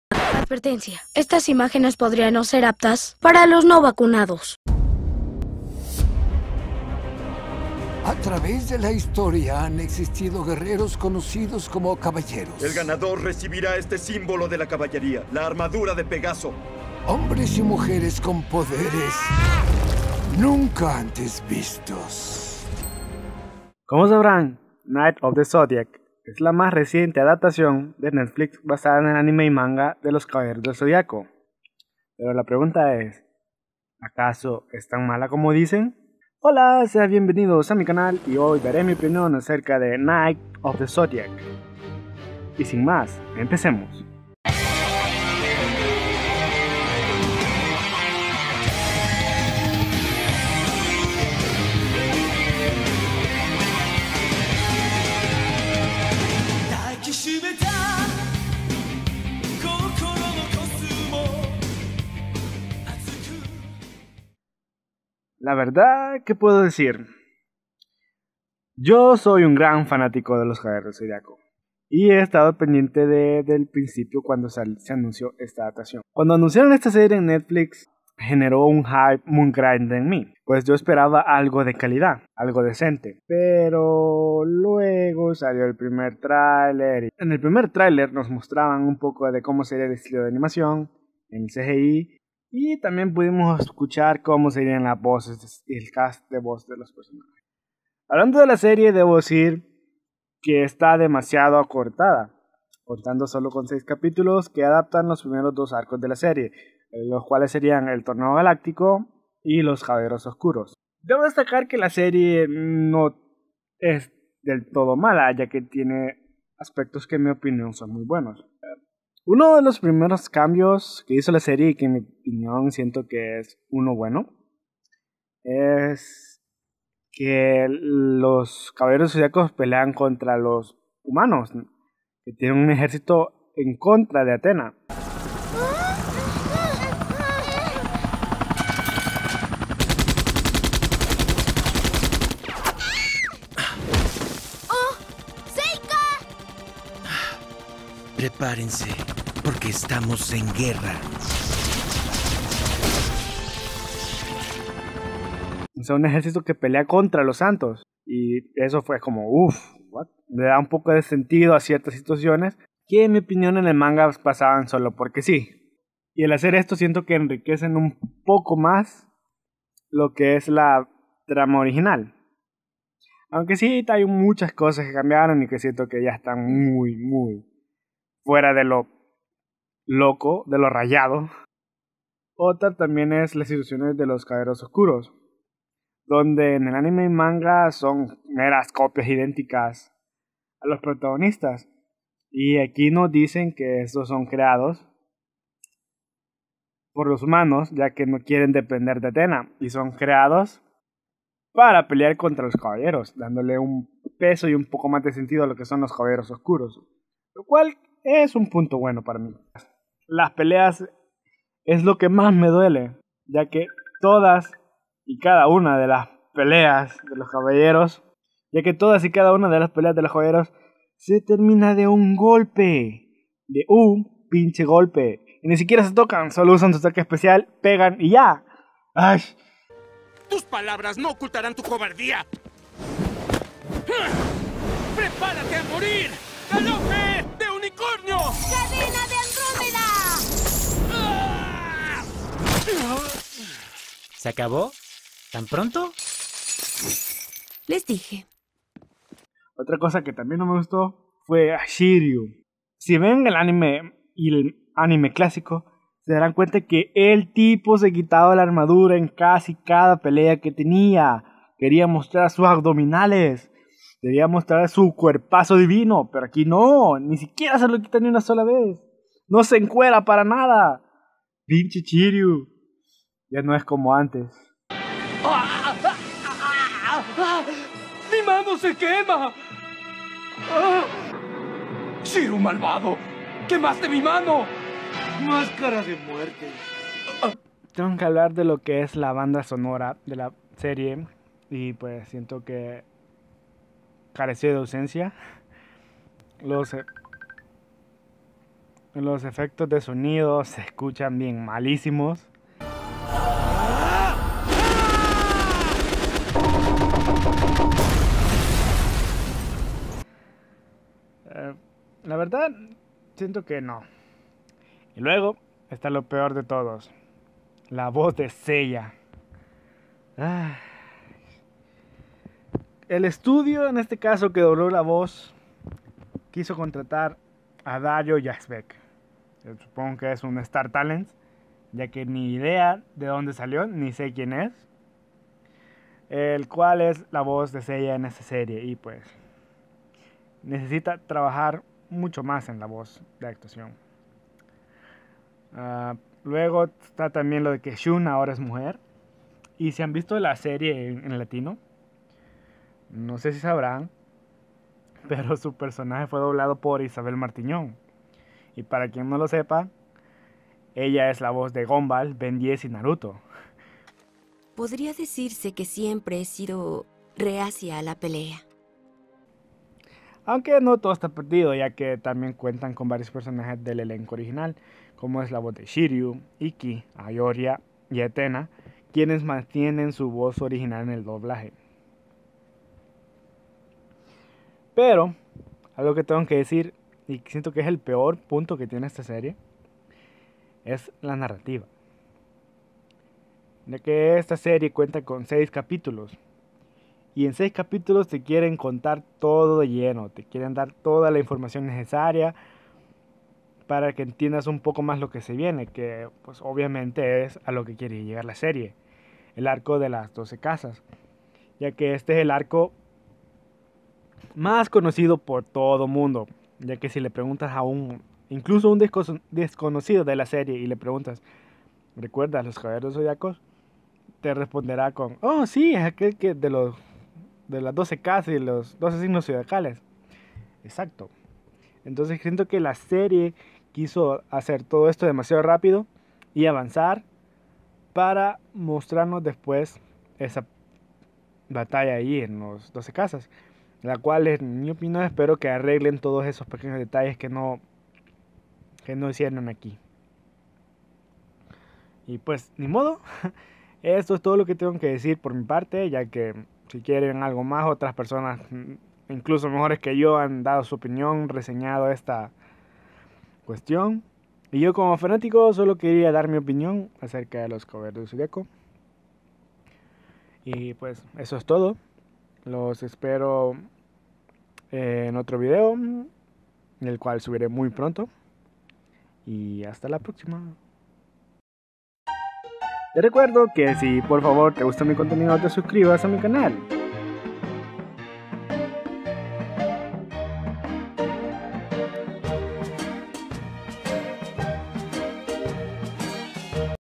Saint Seiya de Netflix |Es Tan mala como dicen??
Opinion de la Adaptacion de Netflix basada en la obra de  Masami Kurumada,  del Mismo Nombre hoy dare mi Opinion si es Mala o si es buena?